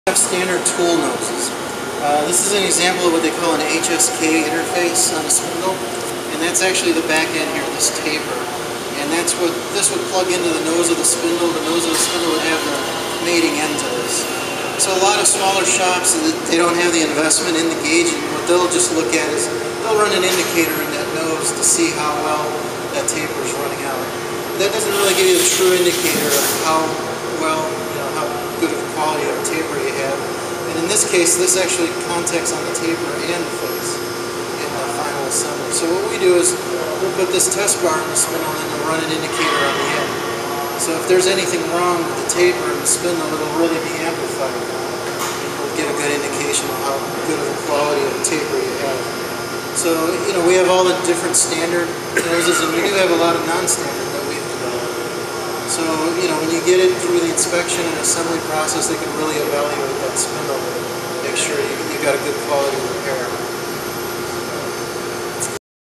HSK Interface
Cvitkovic describes the importance of an accurate and quality spindle taper.